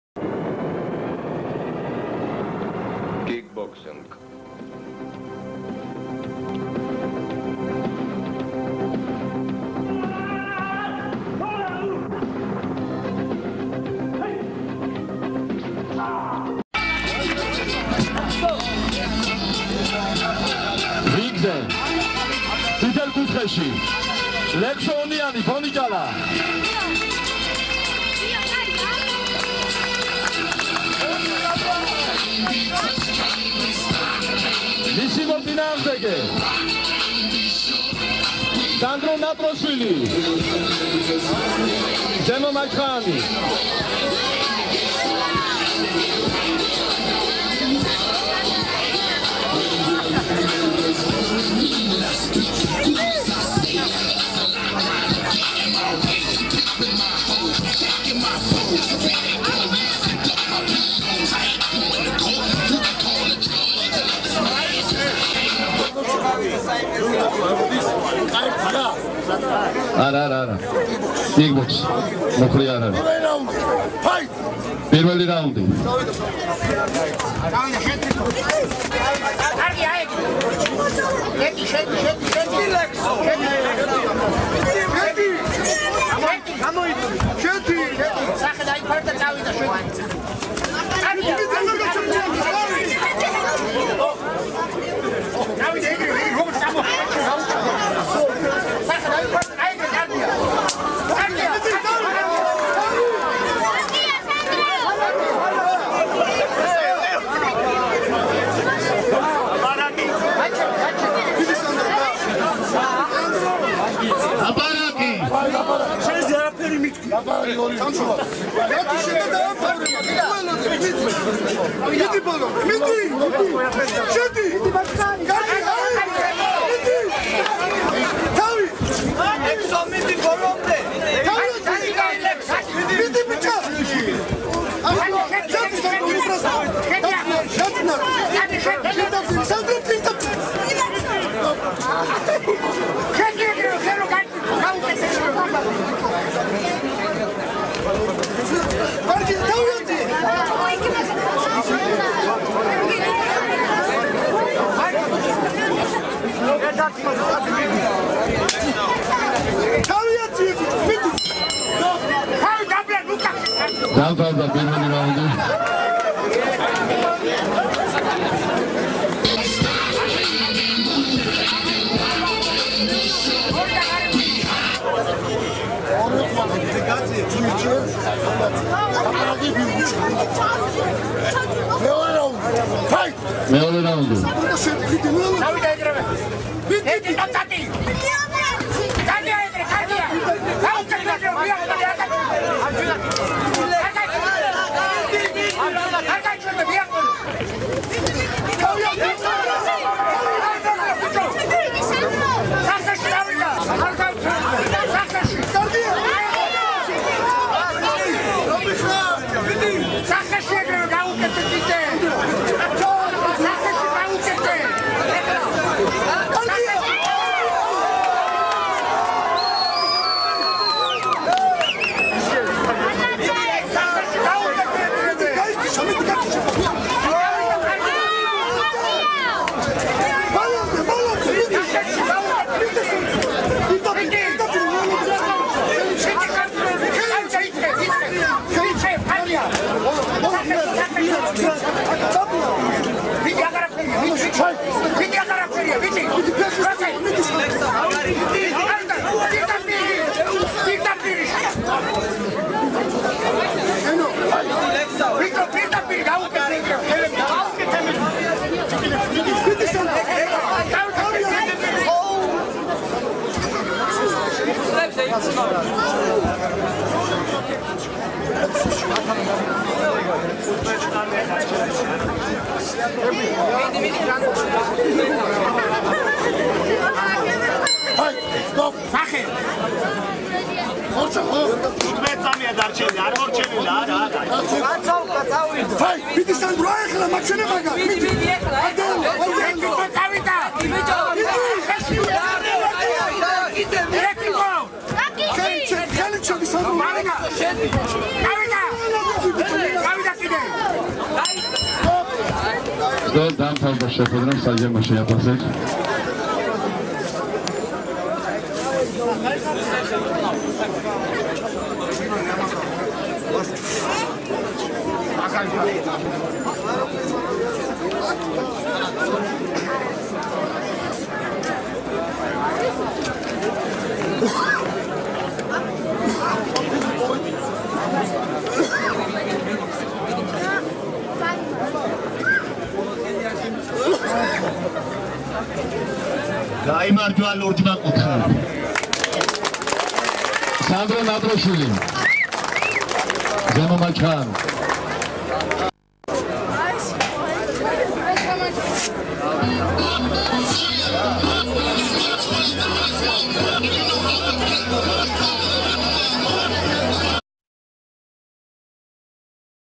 kick-boxing ARQIVI 79 05.06.2016W.REGION.TURN(WNORI)KLUBI,,KONDORI"(ponichala)TRENER DAVID DATUSANI.
ARQIVI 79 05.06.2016W.REGIONALURI TURNIRI (WNORI) LEQSO ONIANI VS SANDRO NATROSHVILI.KLUBI,,KONDORI"(ponichala)TRENER DAVID DATUSANI.